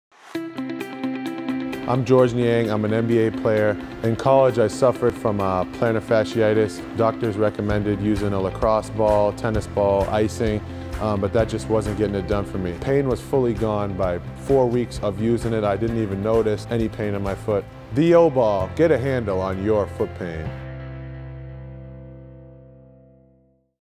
NBA Player Shares How He Finally Got Relief From Plantar Fasciitis (Short video)
"The Oh Ball is amazing and gave me a pain feet for the second half of the season." - Georges Niang, NBA Player

You shouldn’t have to live with foot pain!

The secret to foot pain relief is the OH Ball. The magic is in the handles. Instead of trying to roll your foot over a hard ball on the floor, the handles give you the ability to target the pain point and solve foot pain for good.

The OH Ball relieves pain from:
- Plantar Fasciitis
- Heel Spurs
- Stress
- Muscle Soreness

Made from American maple hardwood, naturally hypo-allergenic, anti-microbial, anti-bacterial, organic, and American made military 550-pound paracord handles.